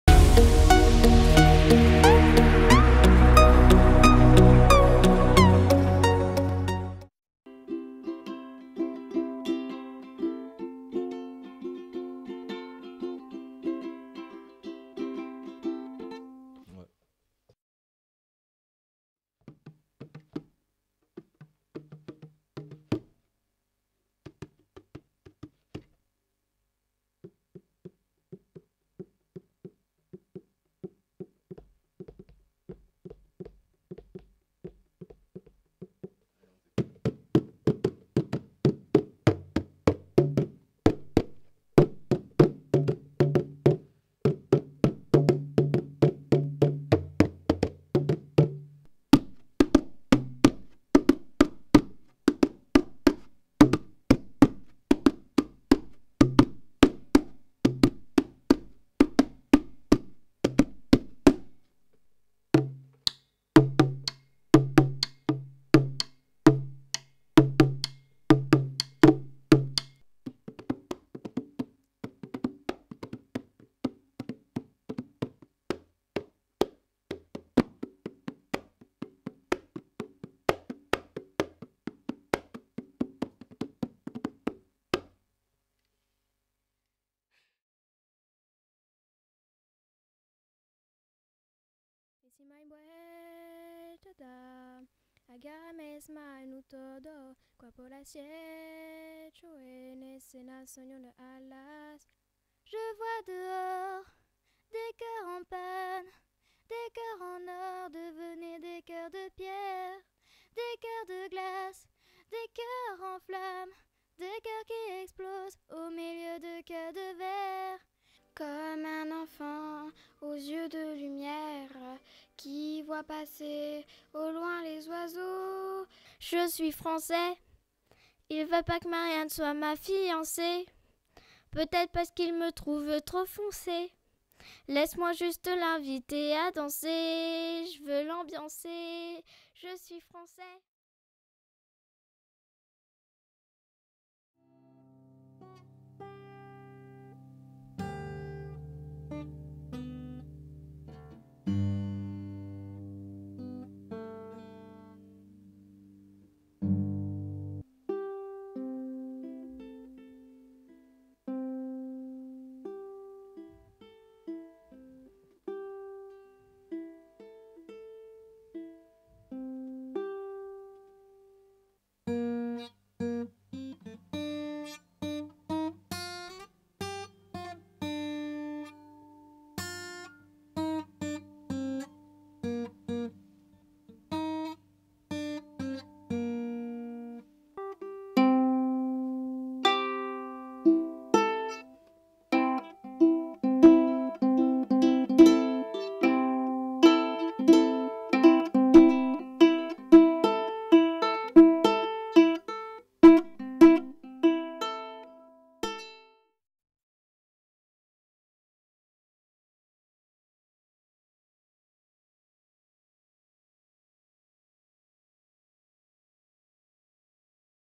Auditions pour intégrer le club musique au collège Henri Morat
Des élèves de 6ème et de 5ème ont passé des auditions au C.D.I du collège Henri Morat afin de participer au club "musique" animé par M.Hugnet.